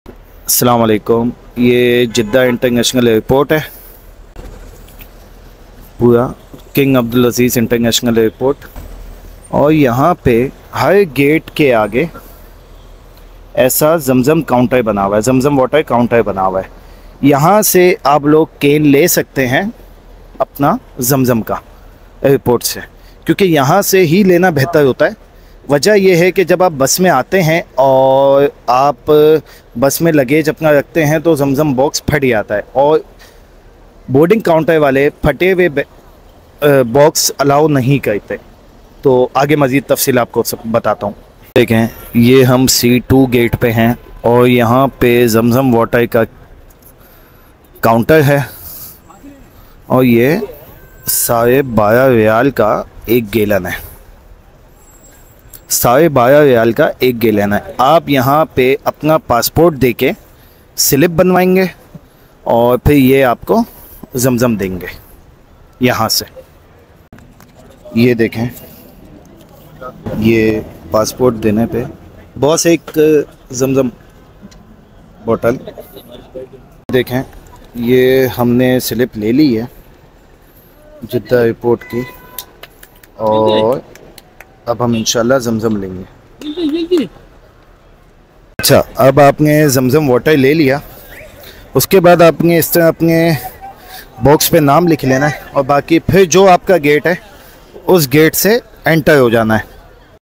How To Buy Zam Zam Water At Jeddah Airport
**Title:** How To Buy Zam Zam Water At Jeddah Airport

**Description:**

💧 **How To Buy Zam Zam Water At Jeddah Airport | Quick Guide** 💧

Planning to bring Zam Zam water back home? Watch our quick guide on how to purchase Zam Zam water at Jeddah Airport. We cover everything from locating the sales point to packing tips for a hassle-free experience.

🔹 **Highlights:**
- Step-by-step guide to buying Zam Zam water
- Locations of Zam Zam sales points at the airport
- Tips for packing and carrying Zam Zam water safely

💬 **Share Your Tips!**
Have you bought Zam Zam water at Jeddah Airport? Share your experiences and tips in the comments below!

Share your experiences and thoughts in the comments section below. We love hearing from you!